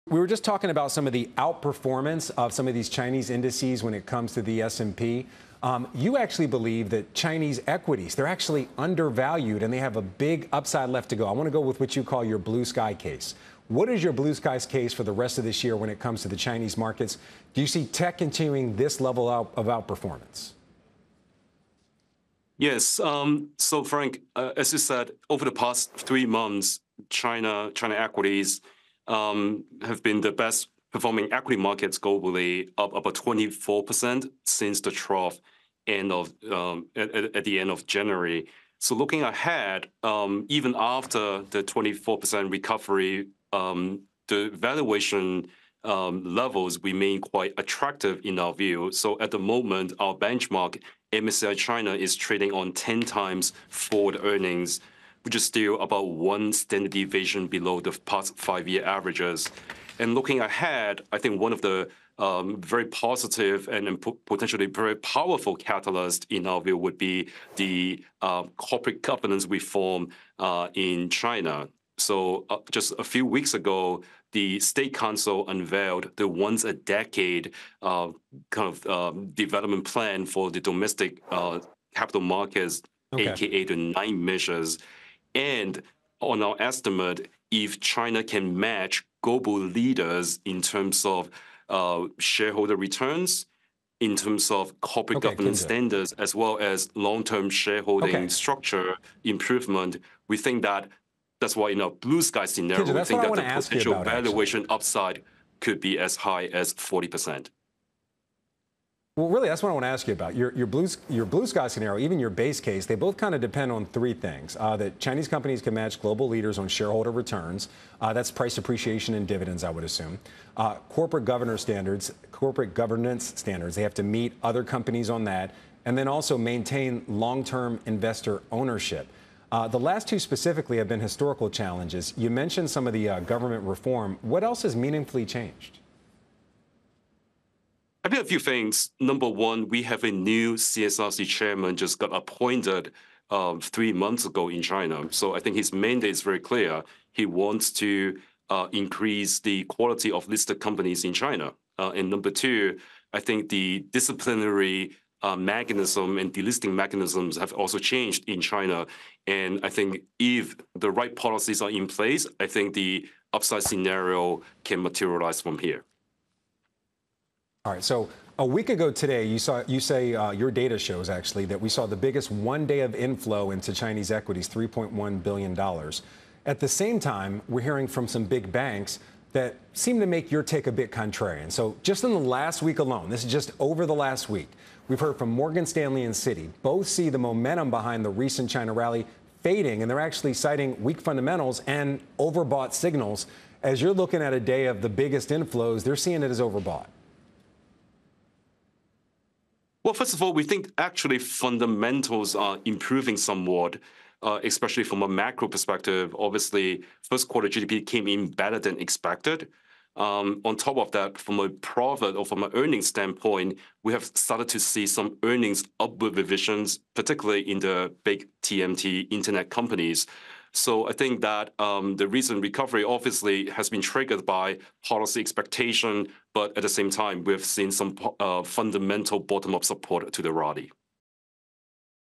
Valuation levels for Chinese stocks are attractive, says Kinger Lau
Kinger Lau, Chief China Strategist at Goldman Sachs, discusses the rally in China stocks over the past two months and where they still have more room to run.